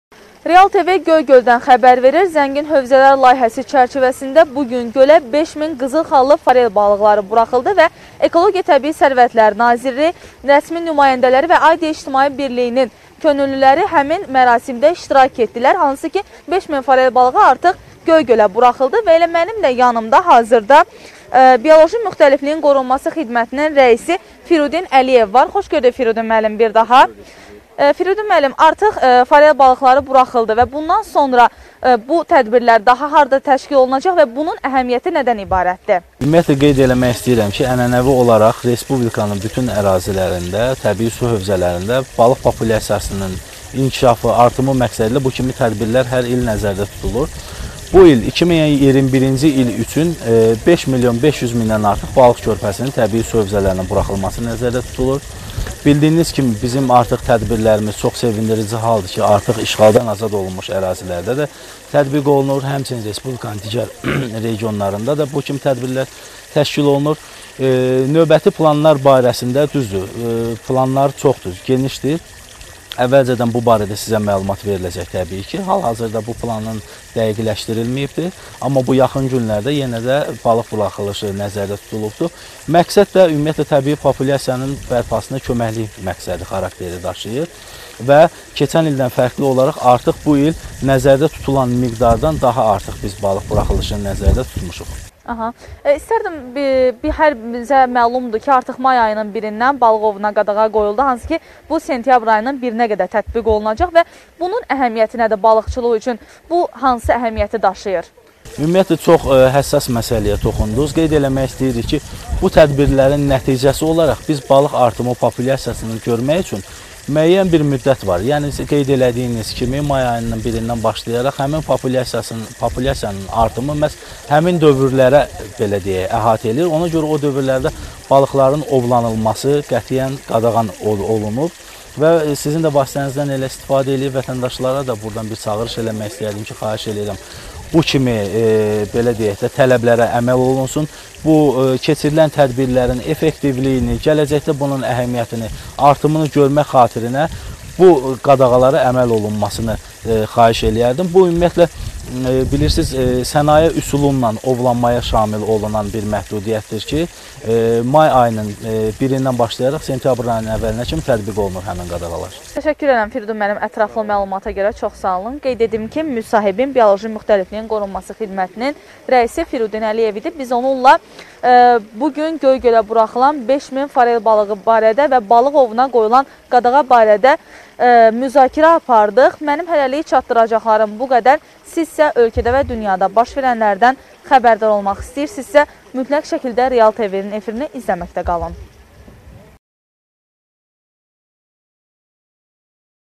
“Zəngin hövzələr“ layihəsi Göygöldə
Gölə 5000 körpə forel balıqları buraxıldı